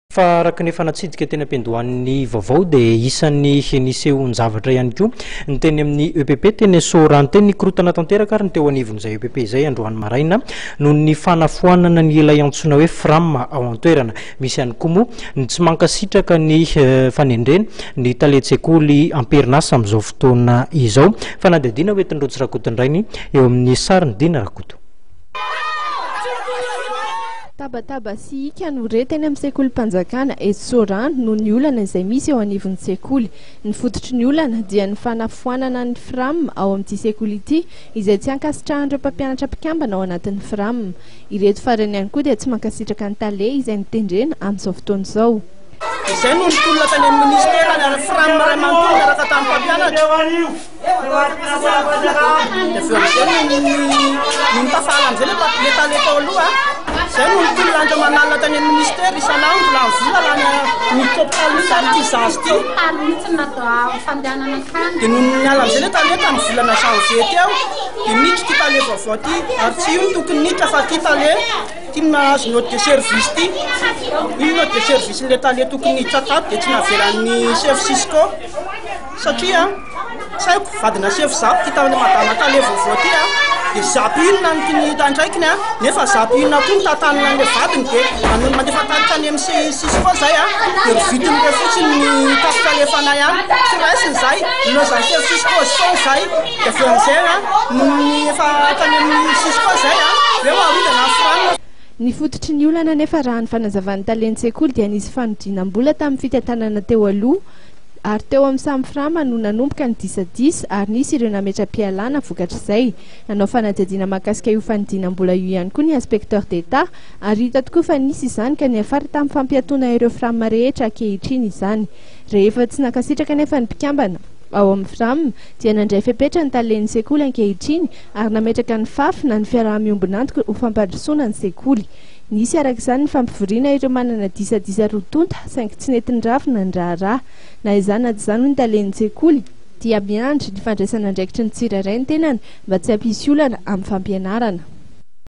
05 EPP SOARANO
Nikorontana tanteraka ny teny amin'ny EPP Soarano androany maraina noho ny tsy fahafalian'ireo mpampianatra sy raiamandren'ny mpianatra, izay tsy manaiky ny hanesorana ny mpikambana ao amin'ny FRAM.